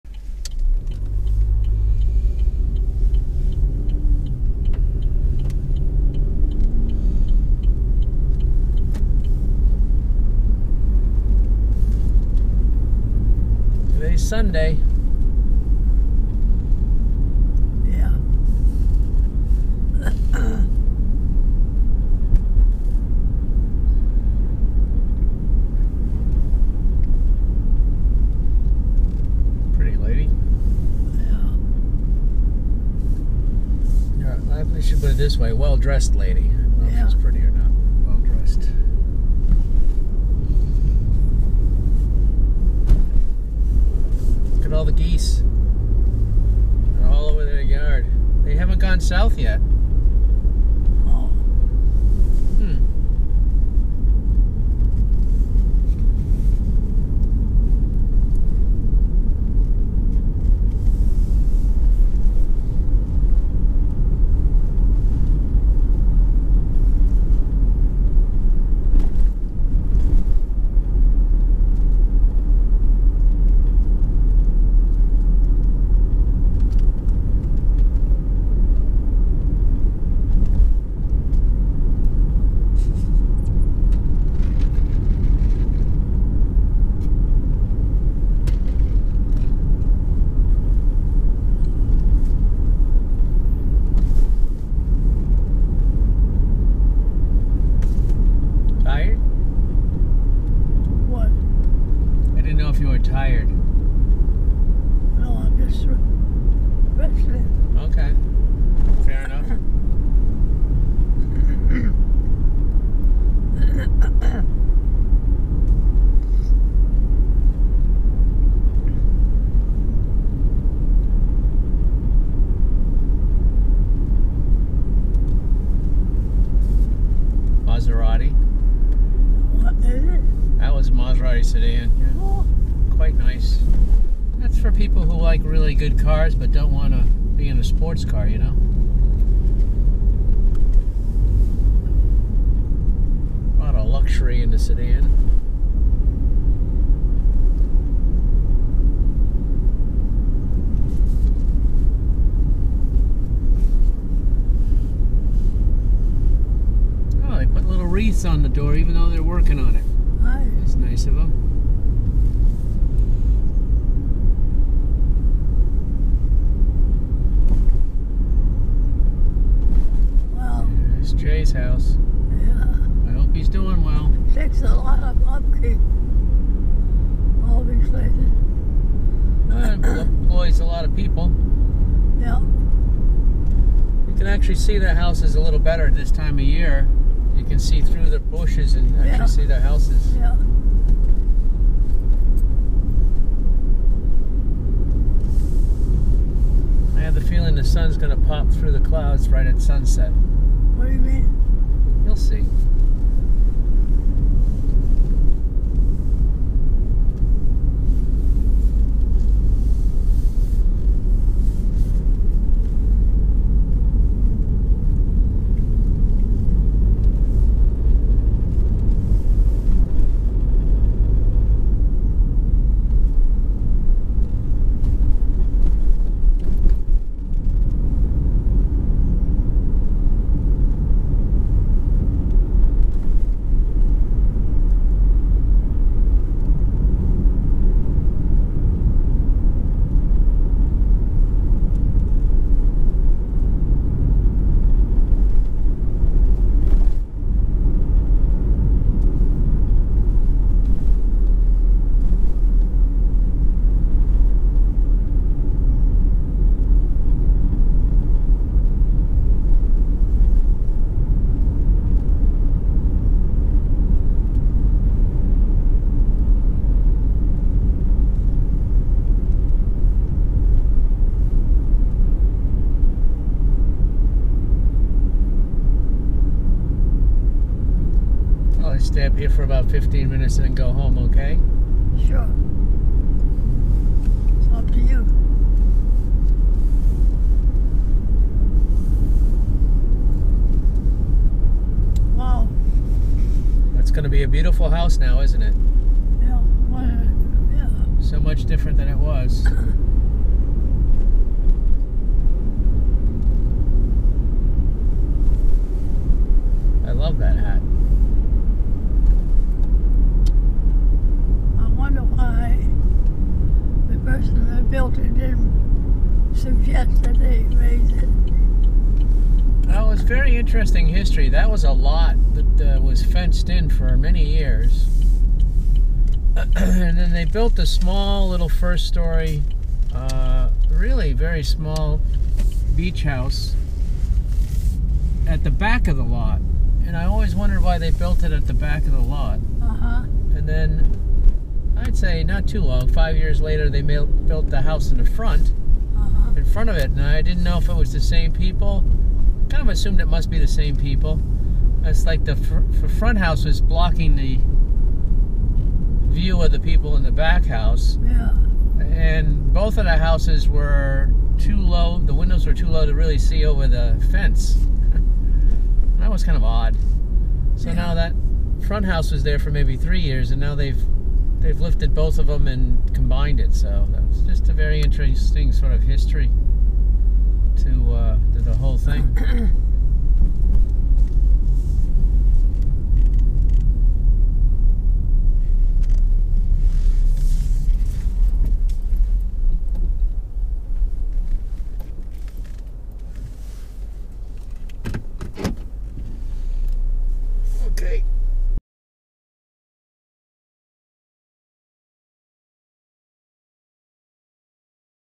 Drive to Brenton Point Ride Along 1-7-23 POV '4k Ride-Along'
The first test video of our Ride Along series . Rough .. but a start . Starting on Bellevue Avenue and along Ocean Drive's Historic Districts. To our favorite stop Brenton Point Park. 'POV 4k Ride-Along'